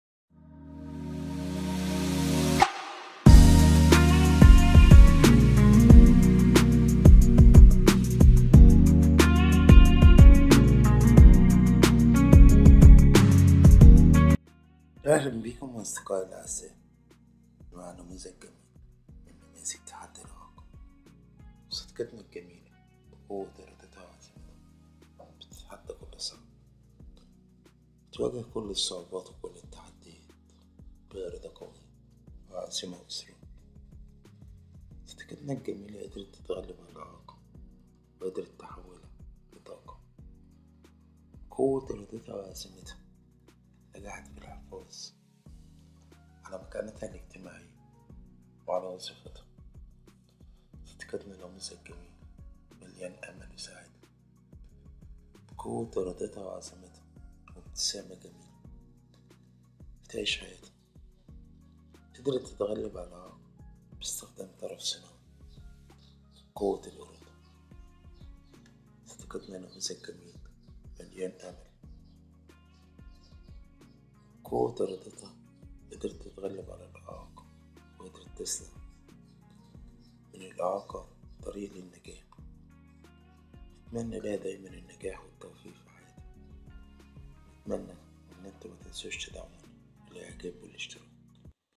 Watch with me how this beautiful woman’s life changed after one of her legs was amputated
Regardez avec moi comment la vie de cette belle femme a changé après l'amputation d'une de ses jambes
Bacağı ampute olan bu güzel kadının hayatının nasıl değiştiğini benimle izleyin
Mira conmigo cómo cambió la vida de esta hermosa mujer luego de que le amputaron una pierna
Veja comigo como a vida dessa linda mulher mudou depois que uma de suas pernas foi amputada